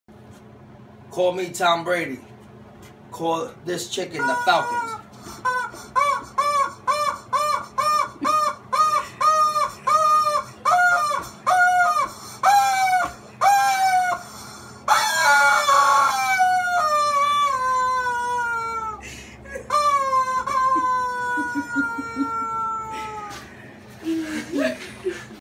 Falcons choked
This is what Tom Brady did to the entire Falcons team on Super Bowl 2017.